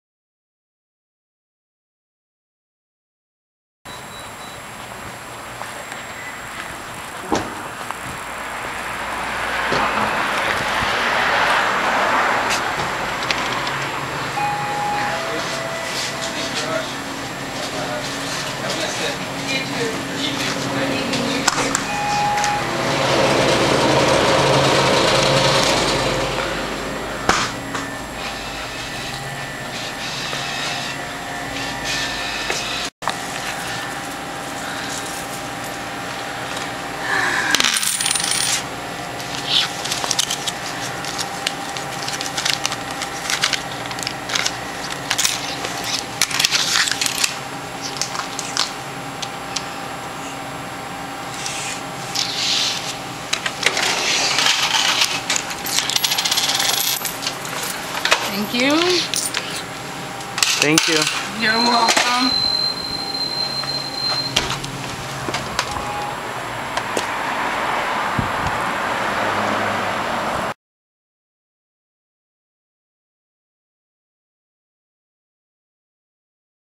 How To Buy A Dr. Pepper...
This detailed video shows how to actully purchase a Dr. pepper. This one was bought at Rotten Robbies Gas station in Santa Cruz Ca on Mission St. Good Luck...